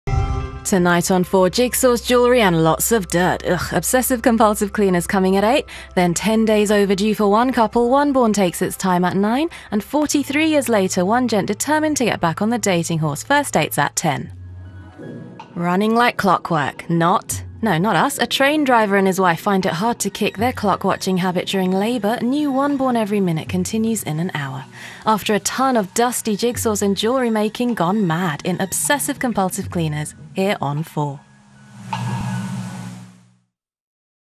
Live Channel 4 Continuity, April 2016
A couple of voice overs by me: Live Tuesday night Channel 4 continuity announcing (5th April 2016). Property of and shared with the kind permission of Channel 4 TV. (Will try and upload more/ a longer demo-reel at a later date.)